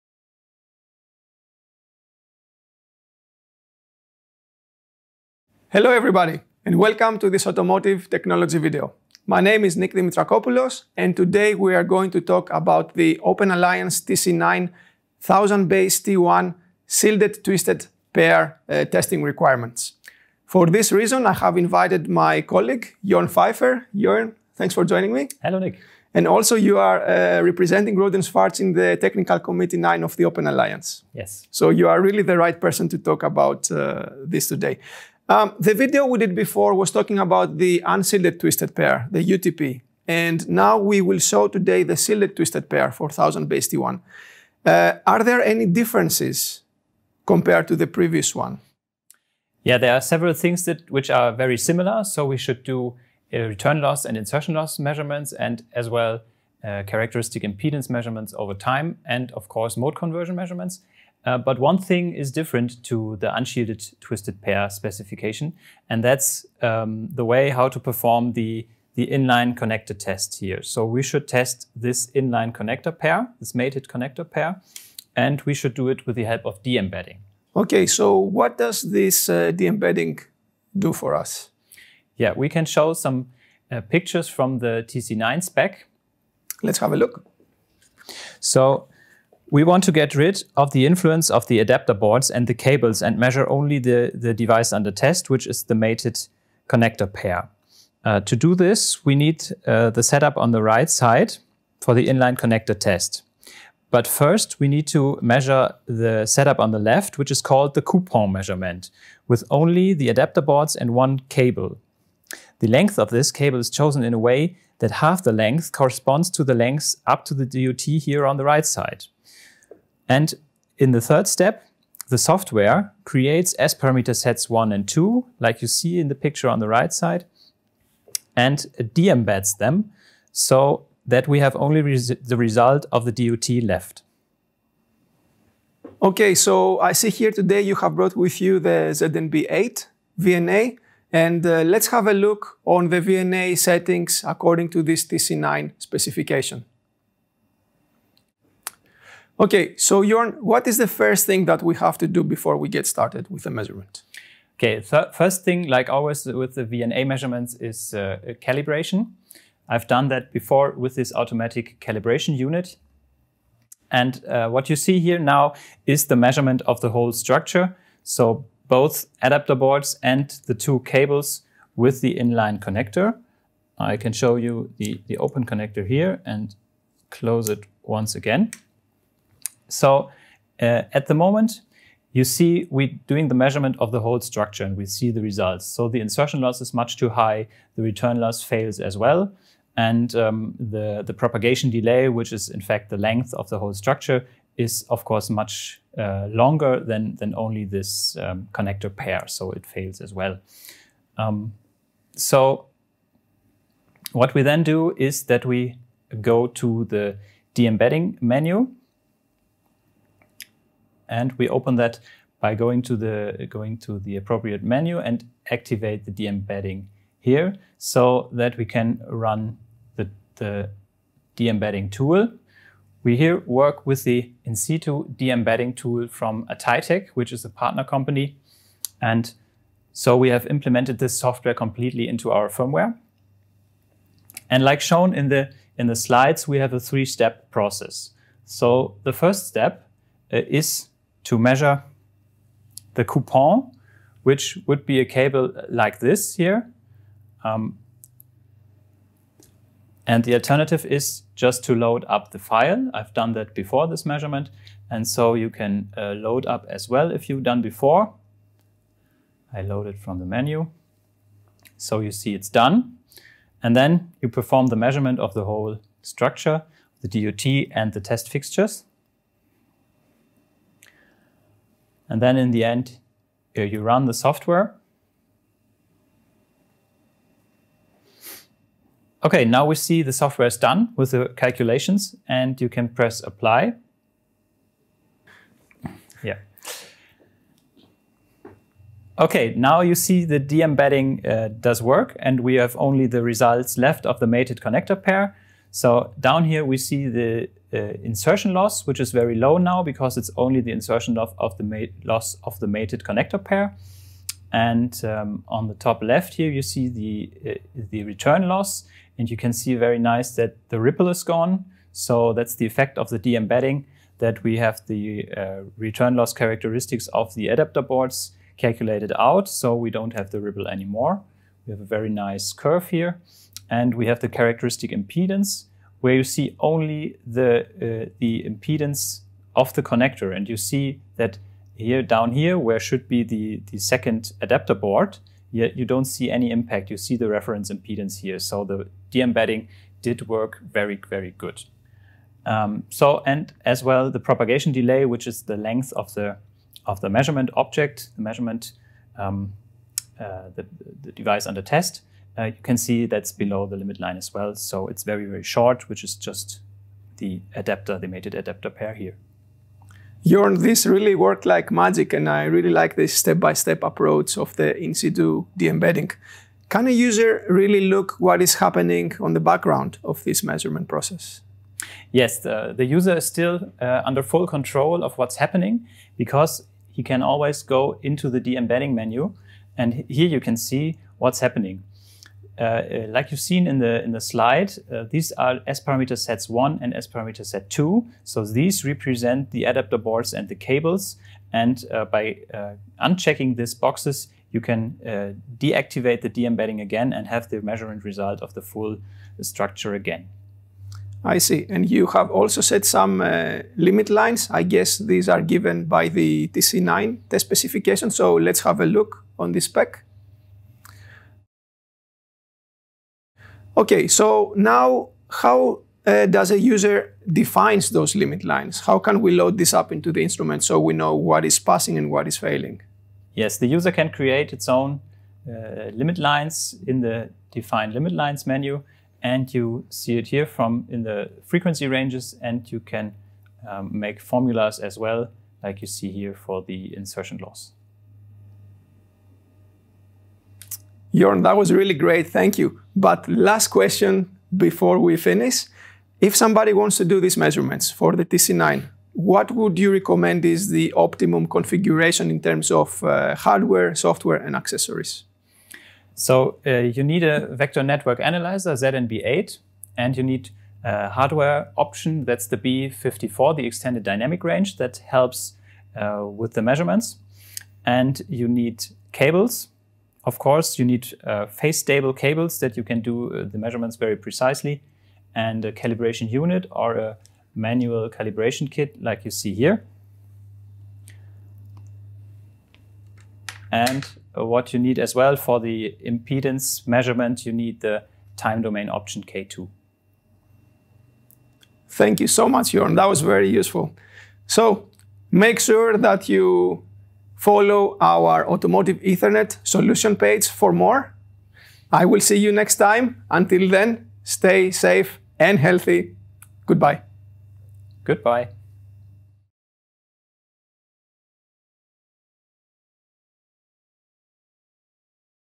Application video: Testing of a 1000BASE-T1 in-line connector
Watch our new application video: Testing of a 1000BASE-T1 in-line connector using the ZNB-K220 In-Situ De-embedding based on the OA TC9 test specification for STP